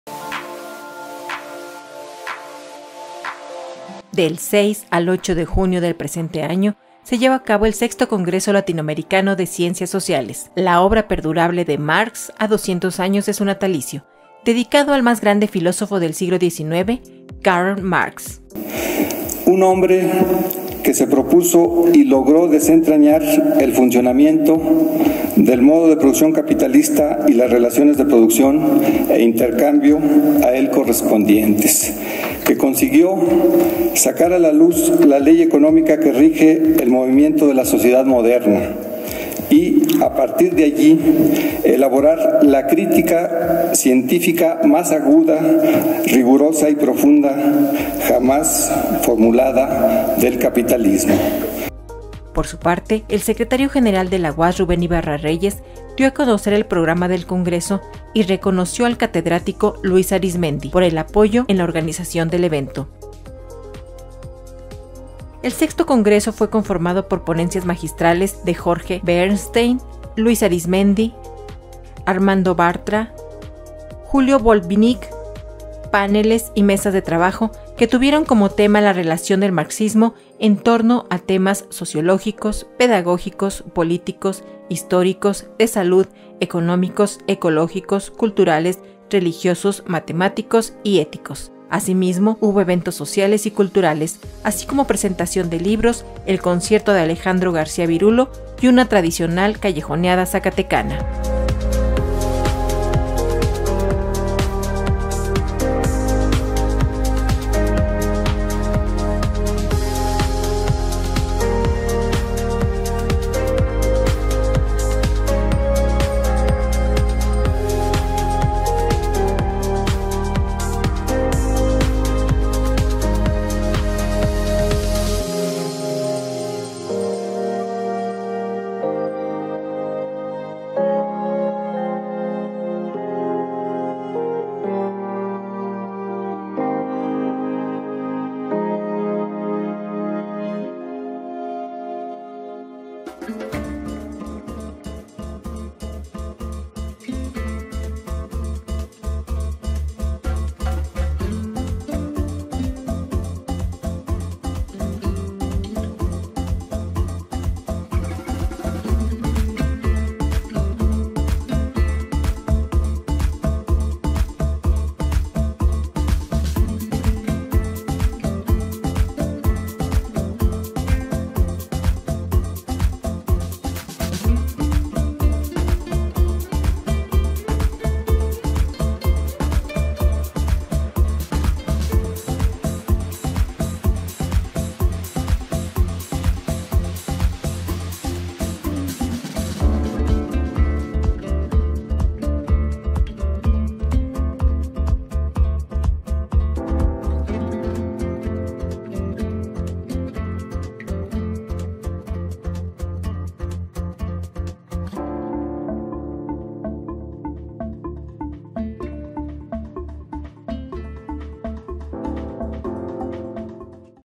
6o Congreso Latinoamericano de Ciencias Sociales 2018
El rector Antonio Guzmán Fernández inauguró el 6to. Congreso Latinoamericano de Ciencias Sociales “La obra perdurable de Marx a 200 años de su natalicio”, dedicado al más grande filósofo del siglo XIX: Karl Marx, que se llevó a cabo de 6 al 8 junio de 2018.

Por su parte, el Secretario General de la UAZ, Rubén Ibarra Reyes, dio a conocer el programa del congreso, y reconoció al catedrático Luis Arizmendi, por el apoyo en la organización del evento.

El sexto congreso fue conformado con ponencias magistrales de Jorge Bernstein, Luis Arizmendi, Armando Bartra, Julio Boltvinik; y páneles y mesas de trabajo que tuvieron como temas la relación del marxismo en torno a temas sociológicos, pedagógicos, políticos, históricos, de salud, económicos, ecológicos, culturales, religiosos, matemáticos y éticos.

Asimismo, y eventos sociales y culturales como presentaciones de libros, el concierto de Alejandro García “Virulo”, y una tradicional callejoneada zacatecana.